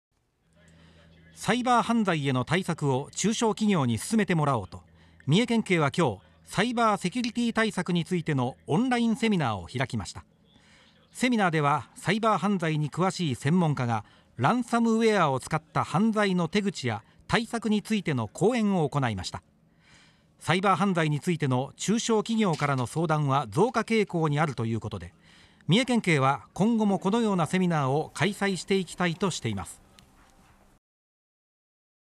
サイバー犯罪の手口や対策 中小企業向けのセミナー開催 三重県警
サイバー犯罪への対策を中小企業に進めてもらおうと、三重県警は3日、サイバーセキュリティ対策についてのオンラインセミナーを開きました。【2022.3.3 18:50放送】